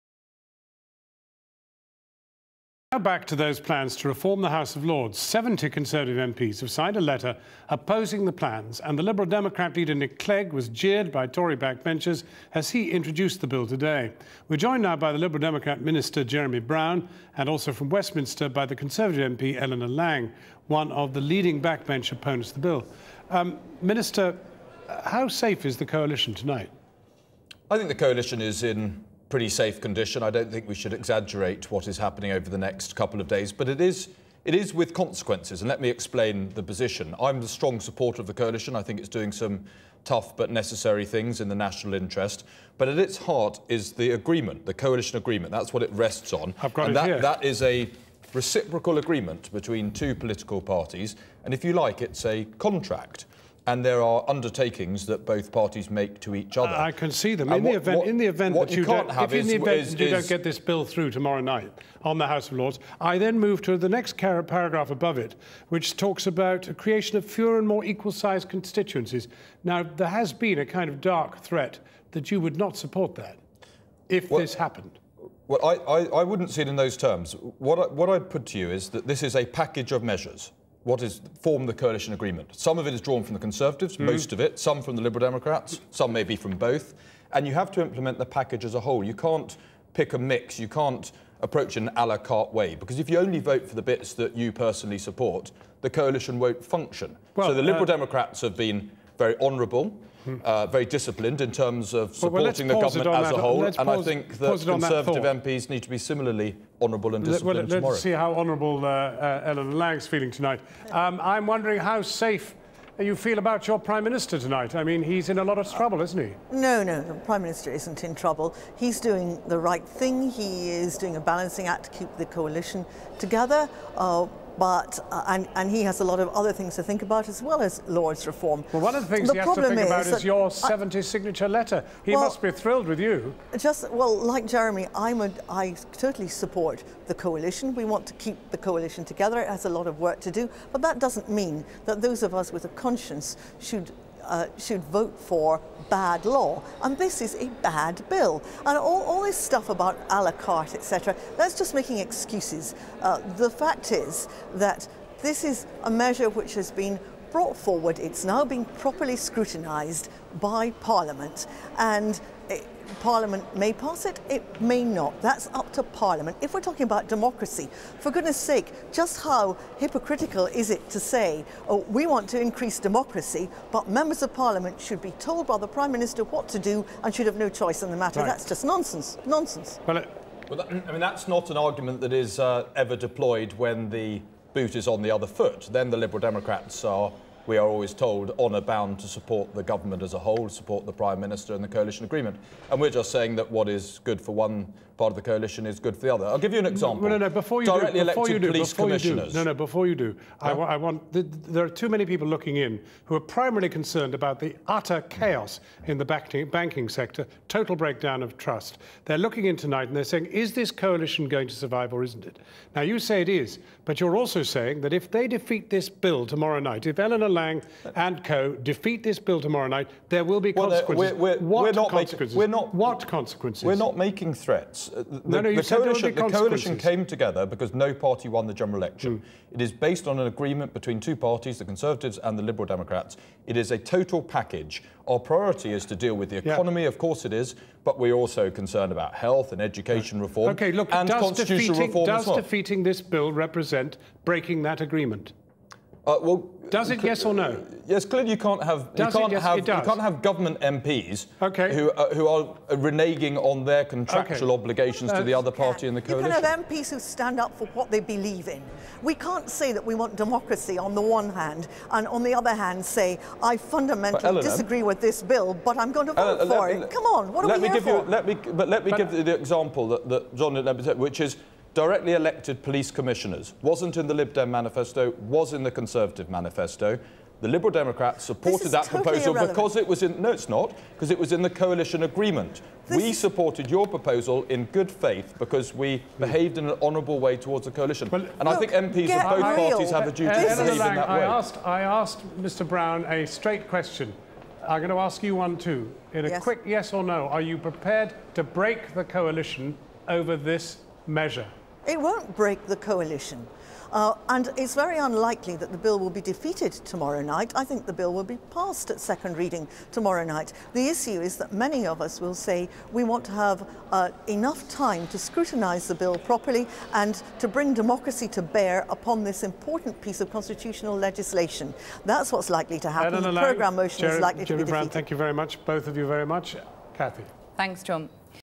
Lords reform: debate
The Liberal Democrat Minister Jeremy Browne and the Conservative MP Eleanor Laing join Jon Snow in the studio to discuss the proposed reforms to the House of Lords.